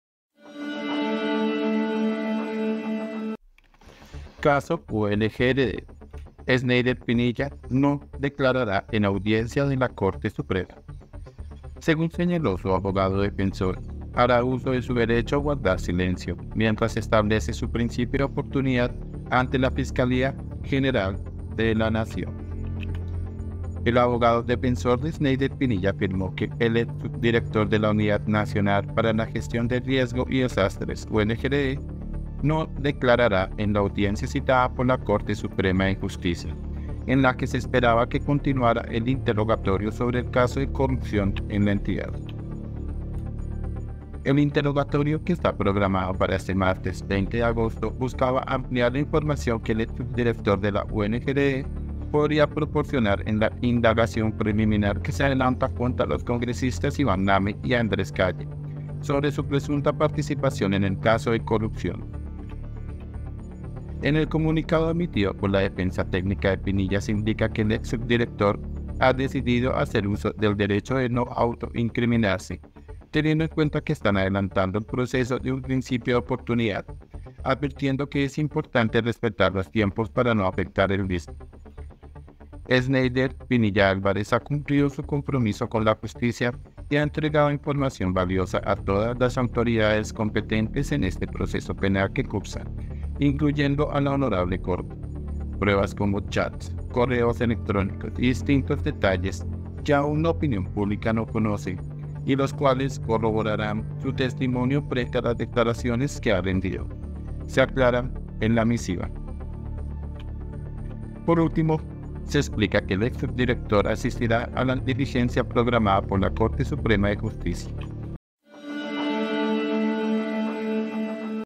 ÚLTIMA HORA CASO UNGRD 🔴 Sneyder Pinilla no declara en audiencia de la C. Suprema
Según señaló su abogado defensor, hará uso de su derecho a guardar silencio, mientras se establece su principio de oportunidad ante la Fiscalía General de la Nación